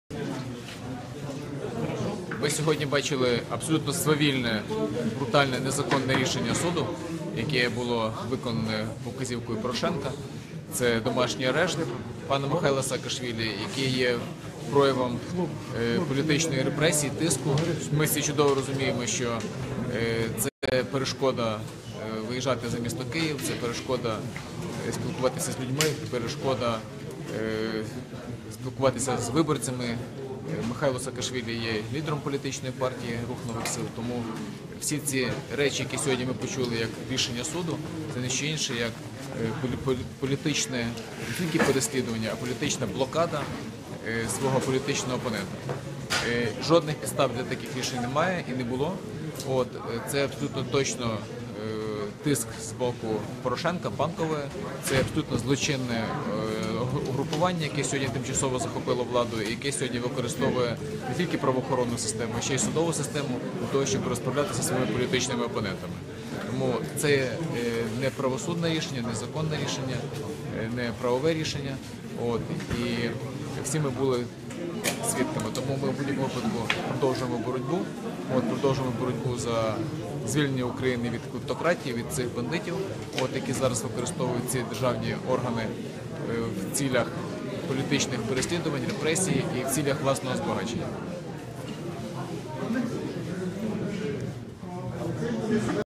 Юрій Дерев'янко: "Нічний домашній арешт по фейковій справі"
Юрій Дерев'янко: "Коментар щодо ганебного судового рішення - нічний домашній арешт -по фейковій справі проти Михайла Саакашвілі".